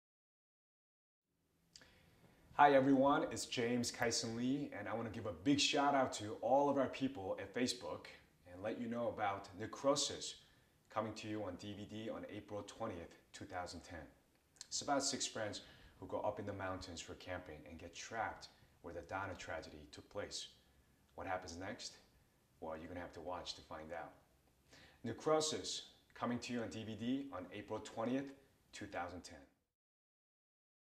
Necrosis Movie - James Kyson Lee does a shout out to his Facebook fans for the upcoming DVD release
James Kyson Lee (Ando on NBC's Heroes) does a shout out to his facebook fans for the upcoming US DVD release of Necrosis on April 20th 2010. Check out the official web page at NecrosisMovie.com and follow along on facebook.